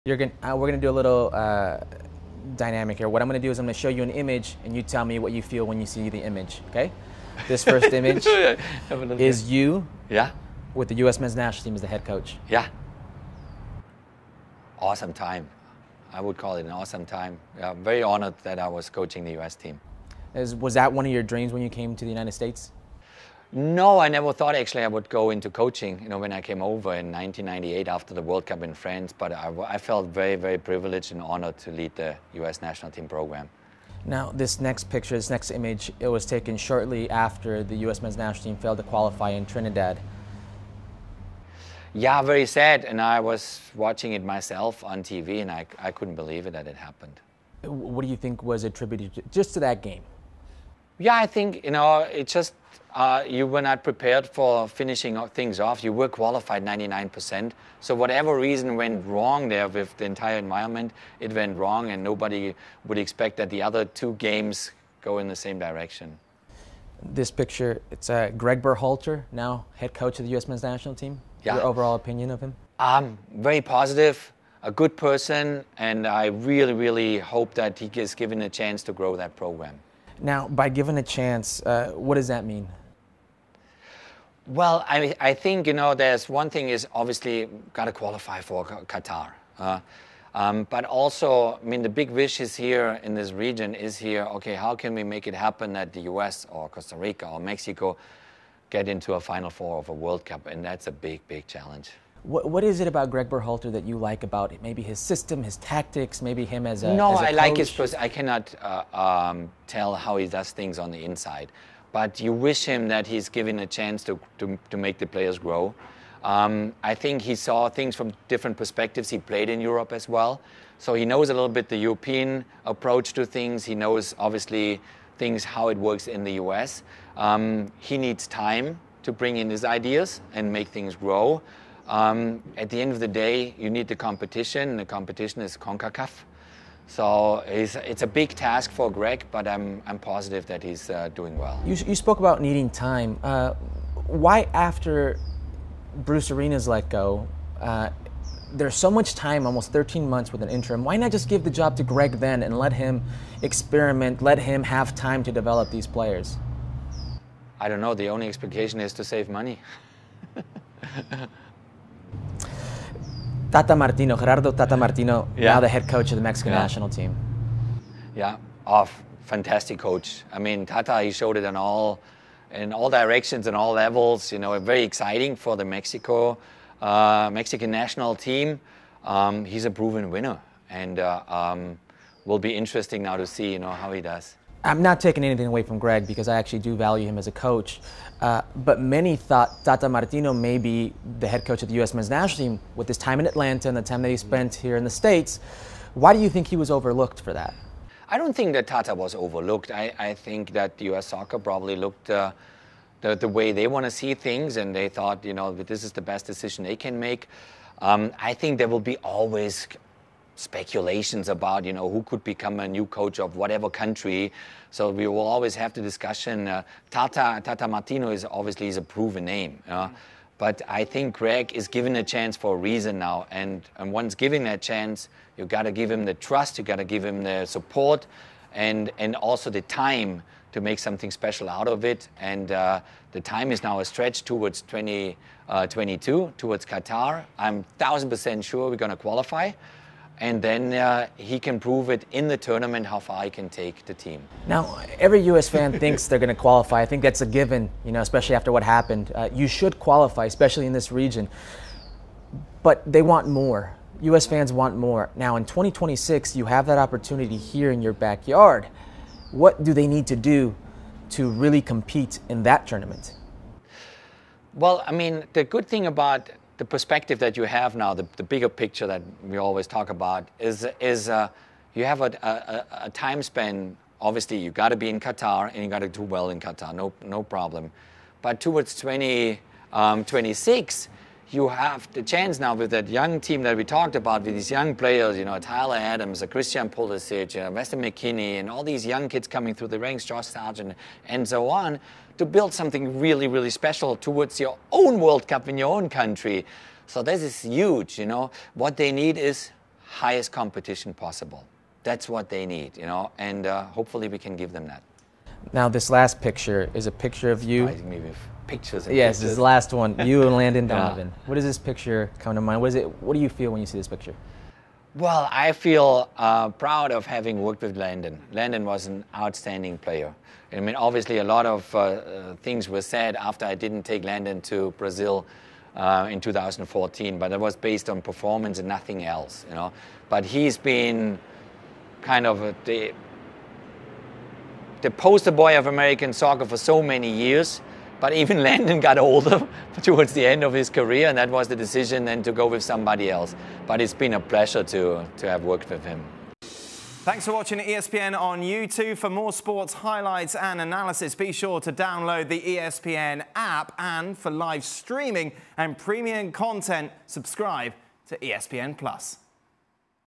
Jurgen Klinsmann revisits decision to leave Landon Donovan off 2014 USMNT World Cup squad | ESPN FC
During an exclusive interview with ESPN FC, Jurgen Klinsmann shares his thoughts on (0:13) his time as coach of the U.S. men’s national team, (0:33) the USMNT’s failure to qualify for the 2018 FIFA World Cup, (1:06) U.S. coach Gregg Berhalter, (2:21) why U.S. Soccer waited more than a year after Bruce Arena stepped down to hire Berhalter, (2:40) Mexico coach Gerardo “Tata” Martino and (5:05) his infamous decision to exclude Landon Donovan from the 2014 World Cup squad.

For more on ESPN FC's exclusive interview with Jurgen Klinsmann, visit ESPNFC.com.